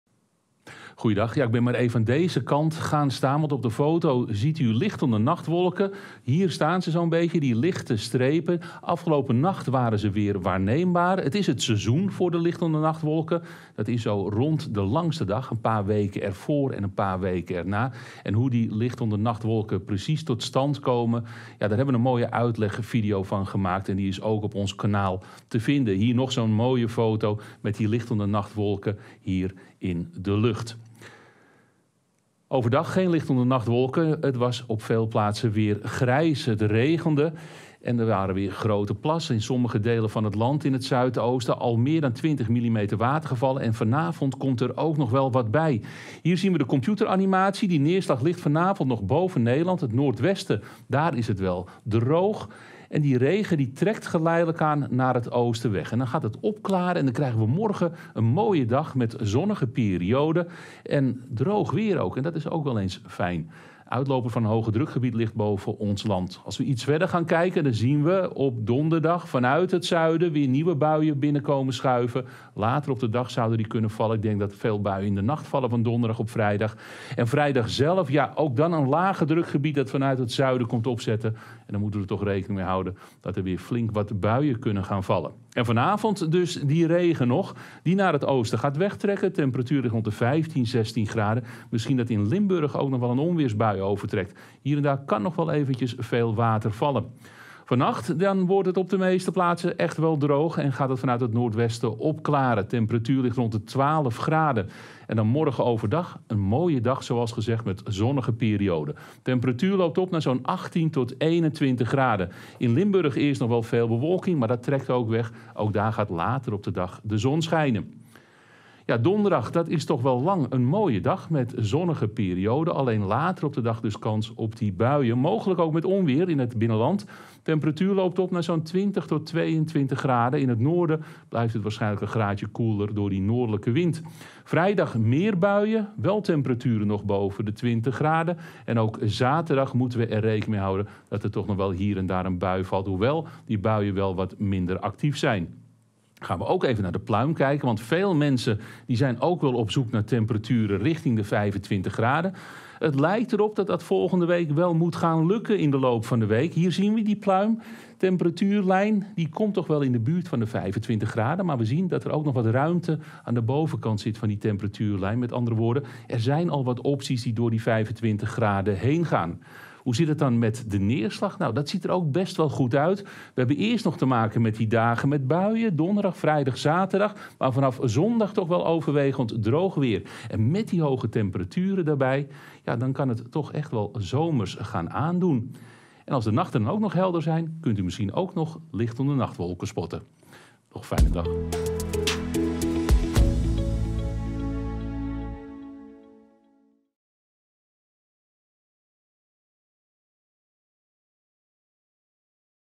Weerbericht: na regen komt zonneschijn
Het weer wordt woensdag een stuk vriendelijker. De zon laat zich zien en het blijft droog. Of het daarna ook zo blijft zie je in ons weerbericht.